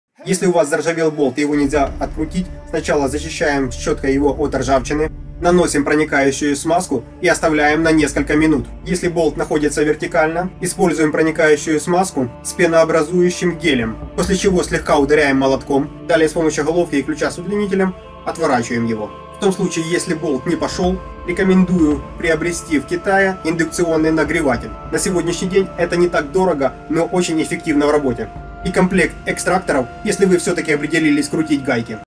Заржавел болт
Если у вас Заржавел болт его нельзя открутить сначала зачищаем щеткой его от ржавчины наносим проникающую смазку и оставляем на несколько минут если болт находится вертикально используем проникающую смазку с пенообразующим гелем после чего слегка ударяем молотком далее с помощью головки и ключа с удлинителем отворачиваем его в том случае если болт не пошёл рекомендую приобрести в Китае индукционный нагреватель на сегодняшний день это не так дорого но очень эффективно в работе и комплект экстракторов Если вы всё-таки определились крутить гайки